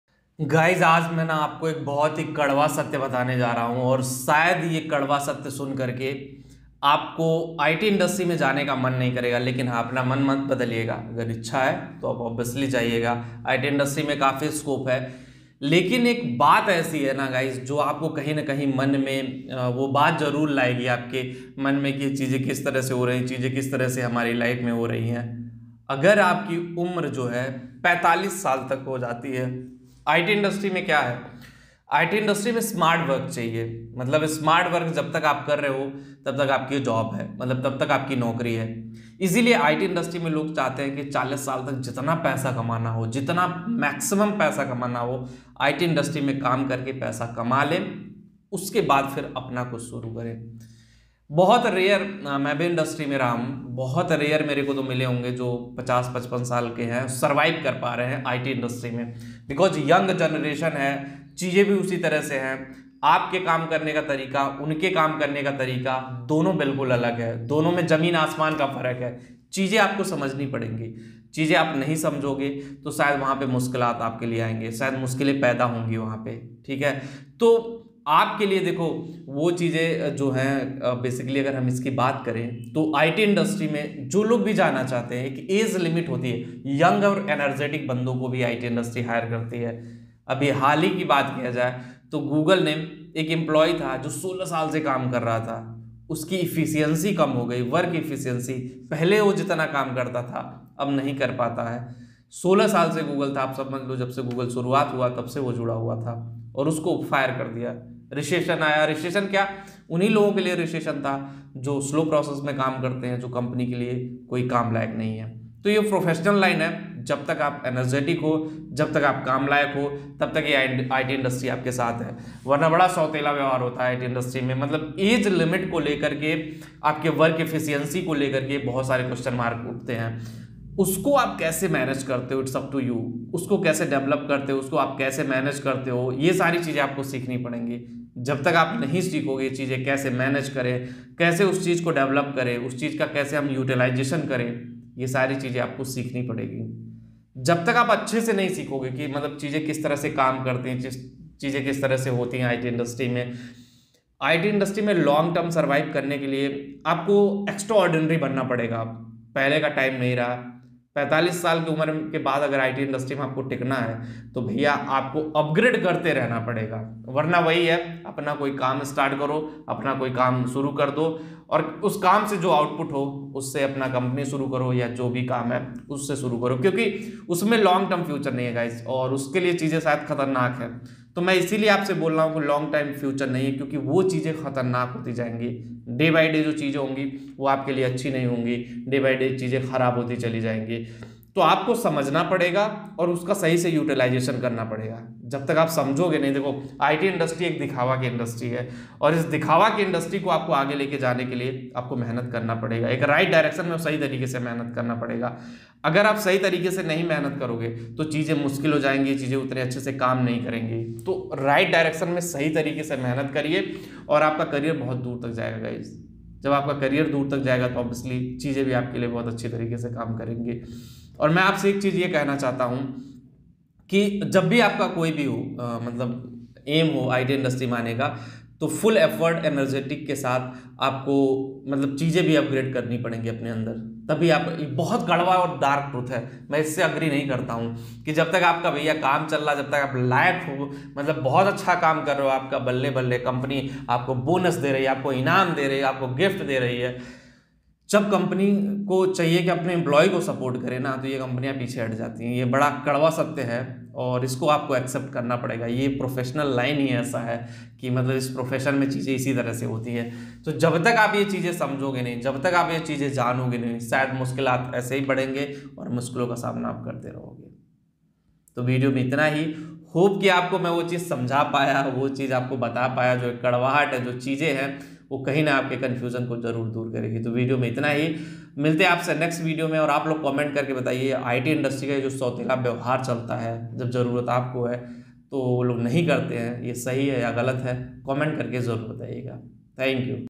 अगर आपकी Age इतनी है तो अब IT कंपनी आपको जॉब नही देगा |ऐसे लोगो का भविष्य बर्बाद है | IT Job Reality
Is video me maine aap sab logo ko IT Industry ke job reality ke baare me bataya hai.